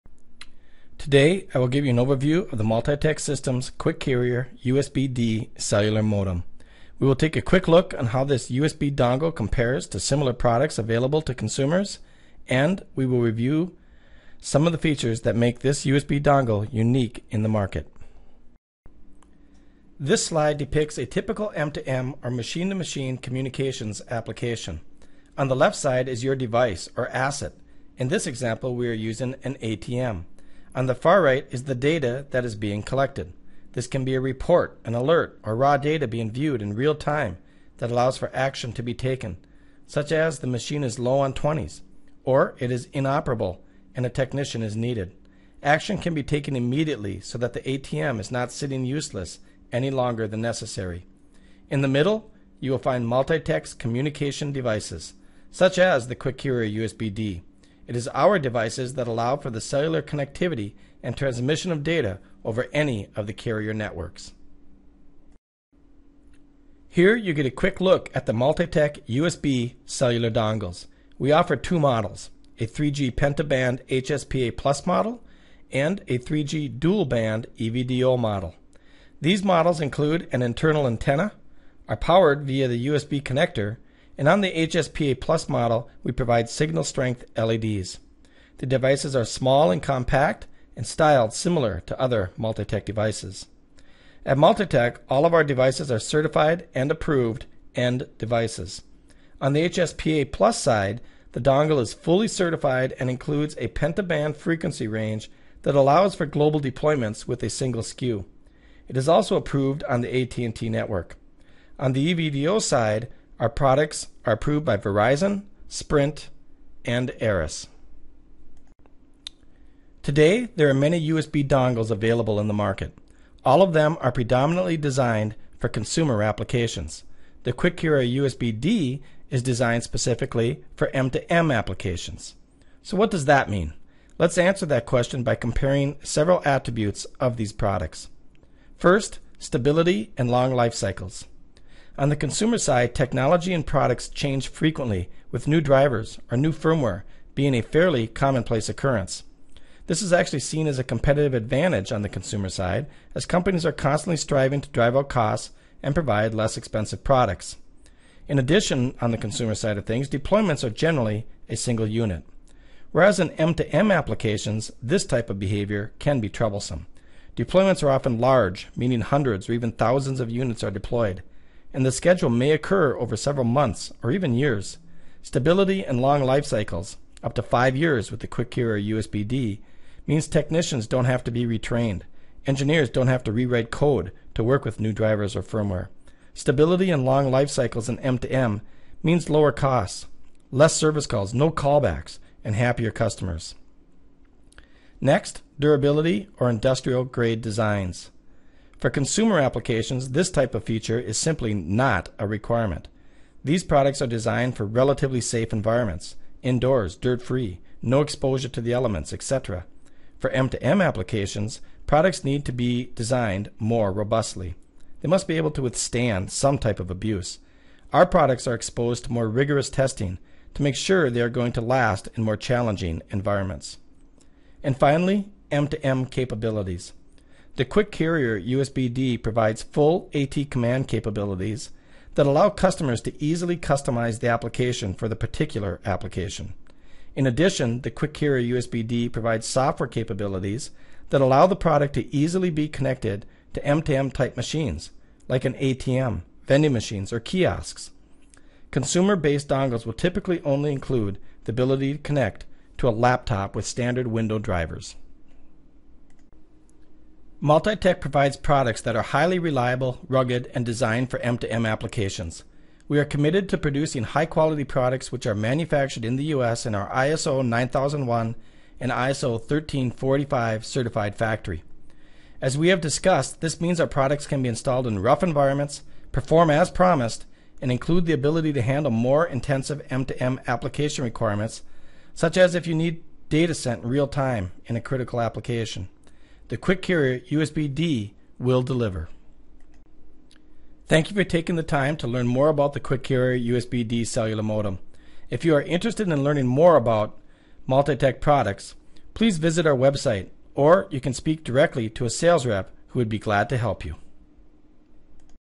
Multi-Tech M2M Device Spotlight: USB Cellular Dongle
The QuickCarrier™ USB-D is a cellular dongle designed and built for M2M (machine-to-machine) applications. That means it is built for long life while delivering reliable data connectivity. The QuickCarrier USB-D is designed for quick implementation of cellular technology -- allowing customers to easily connect their devices to the internet.